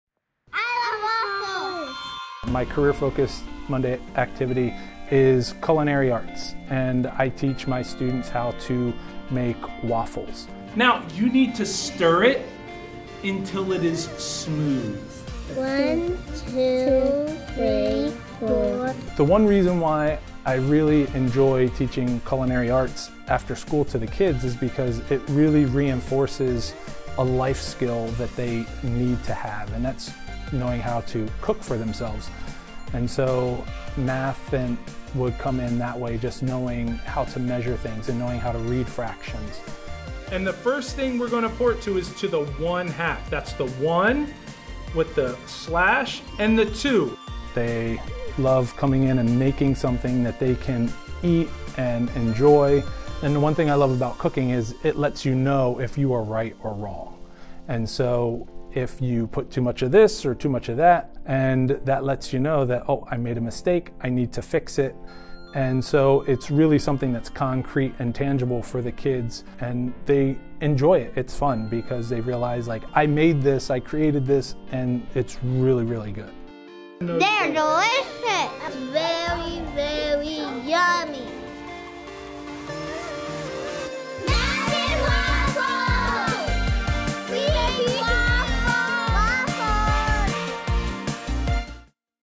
Math and Waffles—Milton Hershey School
Pre-K through fourth-grade students are exposed to careers through the school's Career Monday program where young learners meet with departments across Milton Hershey School and take part in a hands-on learning experience. One MHS teacher is combining math and culinary into a single lesson.